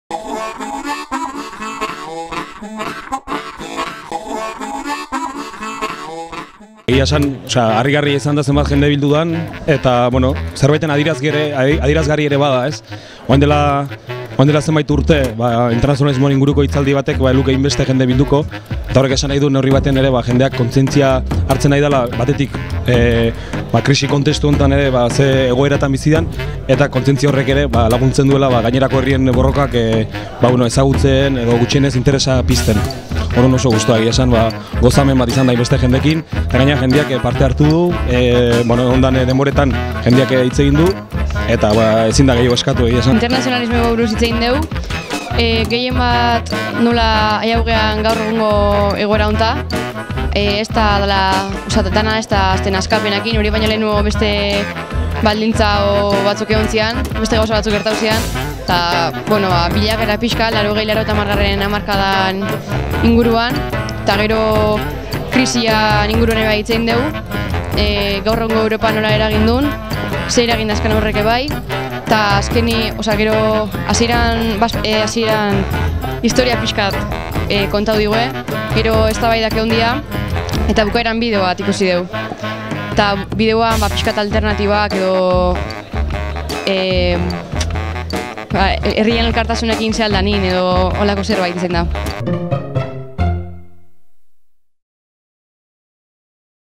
Gazte Danbada / "Hamaika herri, borroka bakarra" mahai ingurua
2013ko martxoaren 29tik apirilaren 1era ospatu da Ernai gazte antolakundearen jaiotza jaialdia: Gazte Danbada.